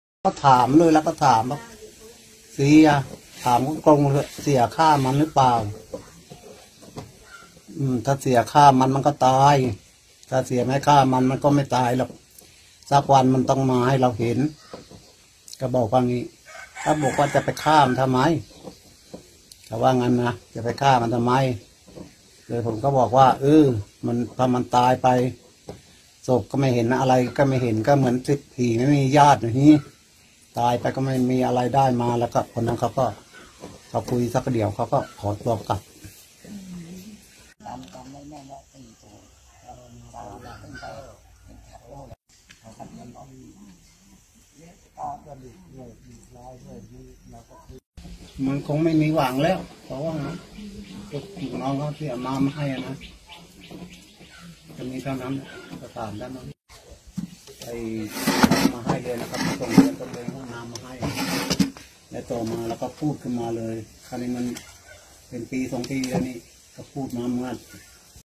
ตา-ยายสาวถูกฆ่าหมกรถจมคลอง เชื่อเสี่ยคนสนิทหลานสาวต้องมีส่วนรู้เห็นการตาย
11/12/2019 ตา-ยายสาวถูกฆ่าหมกรถจมคลอง เชื่อเสี่ยคนสนิทหลานสาวต้องมีส่วนรู้เห็นการตาย

ชัยนาท - ตาและยาย สาวถูกฆ่าหมกรถจมคลอง เชื่อเสี่ยคนสนิทหลานสาวต้องมีส่วนรู้เห็น เผยหลังหลานหายตัวไป เสี่ยมาหาที่บ้านในจังหวัดชัยนาทหลายครั้ง แต่พอถูกถามตรงๆ ว่าฆ่าหลานสาวตนหรือไม่ เสี่ยปฏิเสธและหนีหน้าหายไป